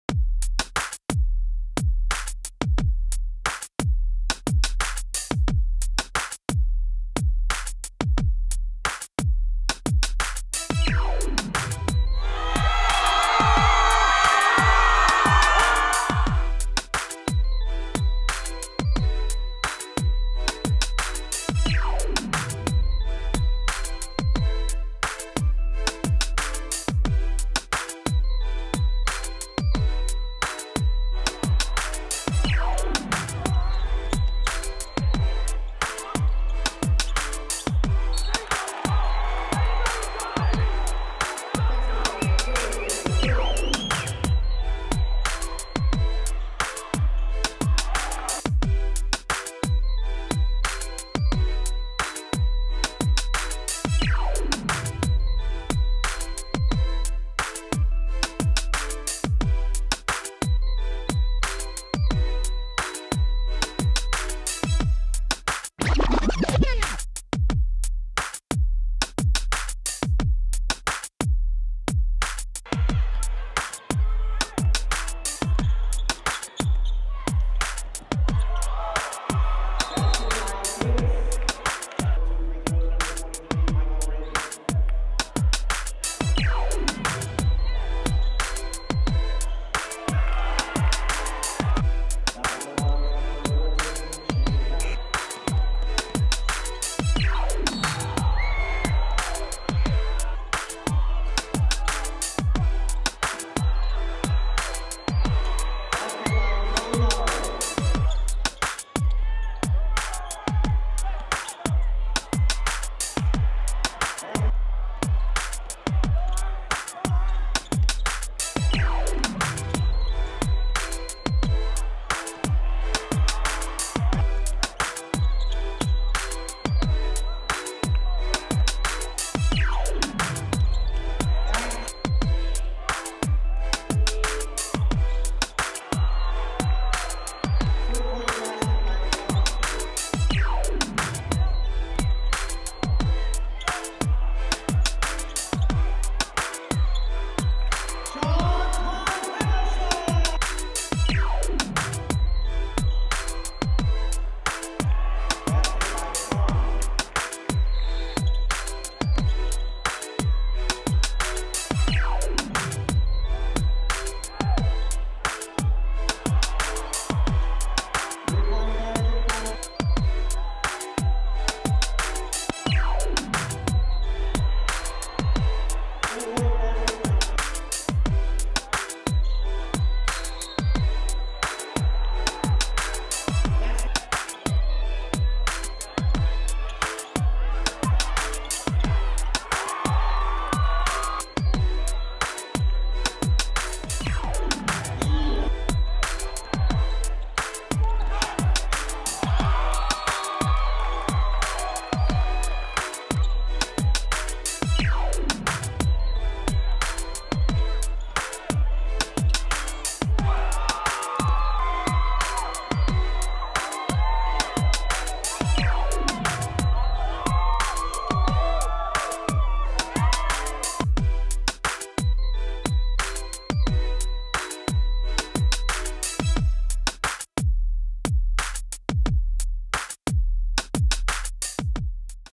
IHSAASports.org Game of the Week - Bloomington South vs Southport - Highlights
Hightlights from the Bloomington South vs Southport game.  After their state finals run last year, Bloomington South begins their season with a strong team and adjusts to the newest additions while taking on a competitive Southport team.